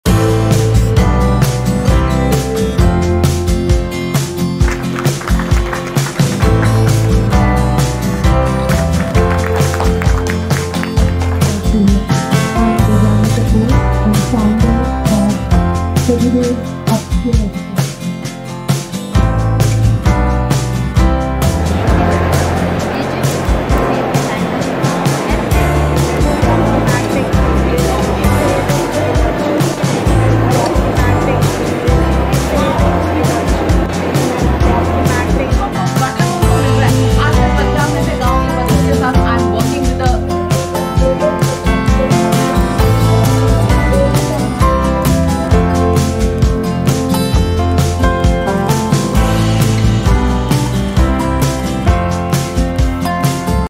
Teachers Conclave | Agra | Edudevs
📚 Empowering Educators: Exploring NEP 2020 & NCF 2023 🎓

 📝Teachers gathered in Mainpuri, Agra, to spark creativity through NEP 2020 and NCF 2023.
We started a journey to deeply explore the revolutionary possibilities of the National Education Policy 2020 and the forward-thinking New Curriculum Framework 2023 at this momentous event. 🌟